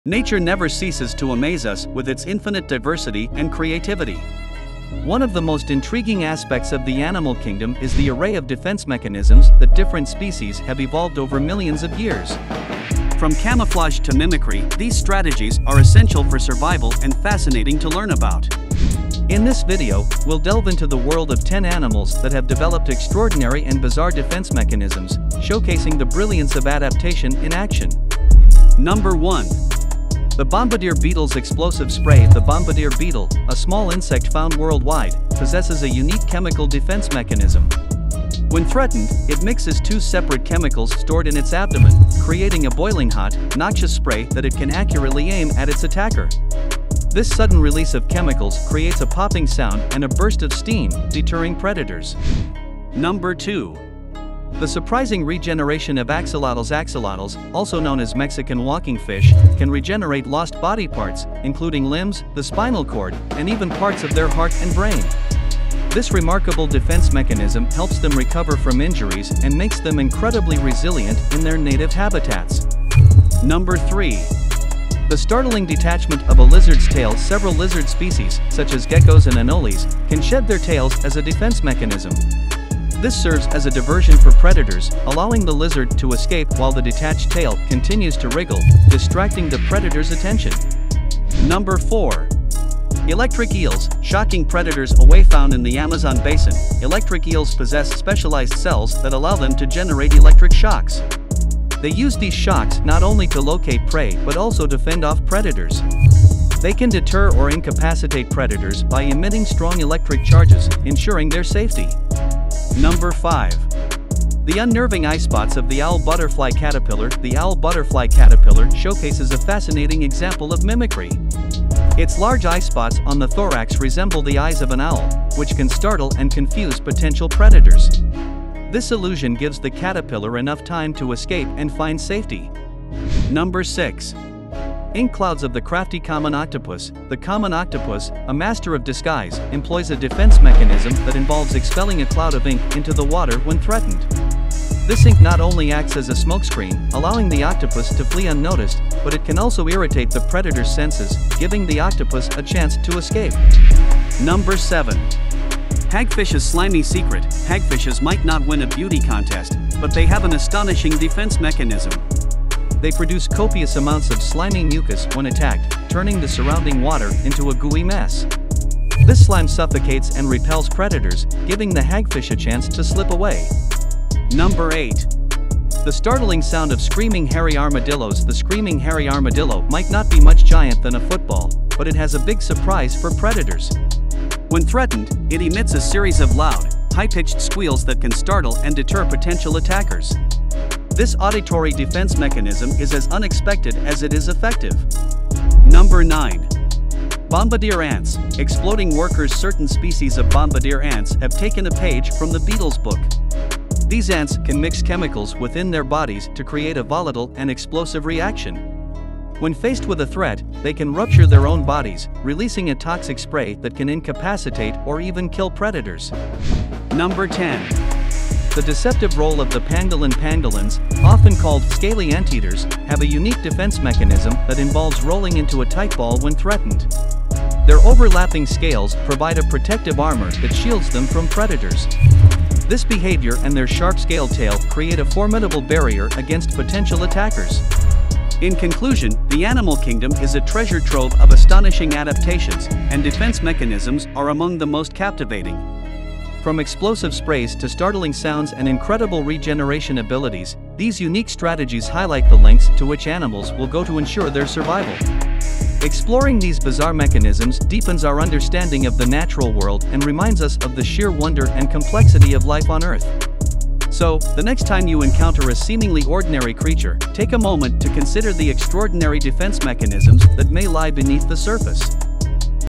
Prepare to Be Amazed: Uncovering the Top 10 Animal Defense Mechanisms
In this video, we're going to take a look at the top 10 animal defense mechanisms in 2023. From the classic fight or flight response to interesting adaptations like slime production, these mechanisms will help animals protect themselves from danger. Whether you're a wildlife enthusiast or just curious about animal defense mechanisms, this video is a great way to learn about some of the weird and wonderful ways animals protect themselves. Watch and learn, and be prepared for when you encounter an animal in the wild!